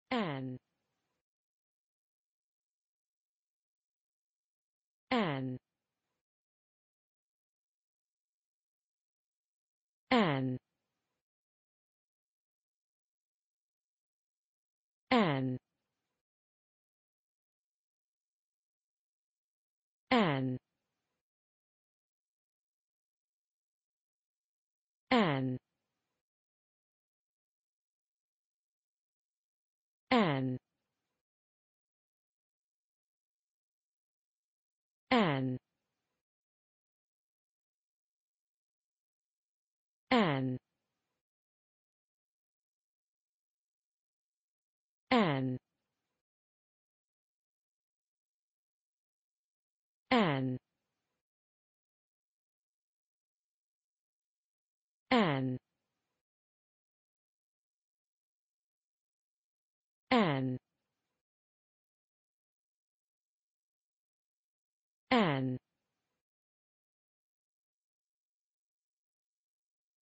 Airan: How to pronounce Airan in english (correct!).Start with A. Learn from me.
Follow me!Learn from me how to pronounce the word Airan in english.This video shows you how to pronounce the word Airan in english.

Airan

Airan is a French commune in the Calvados department in the Basse-Normandie region of northwestern France.